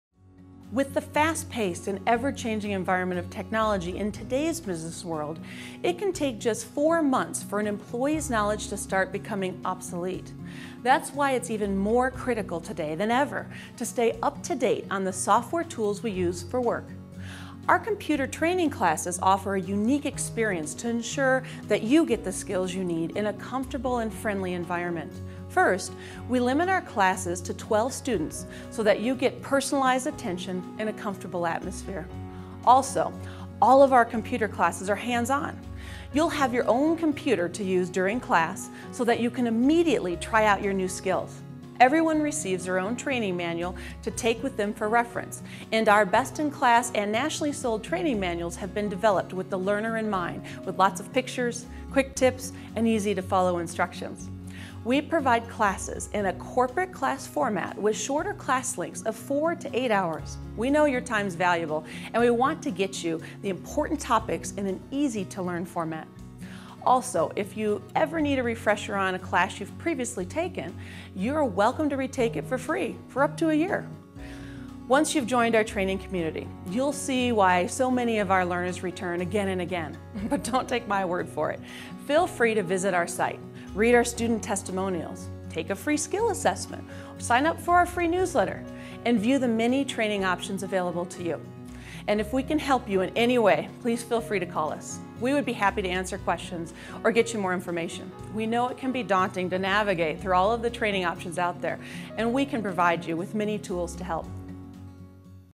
Computer Training - MCS
The value of computer training from Mission Critical Systems in Denver Colorado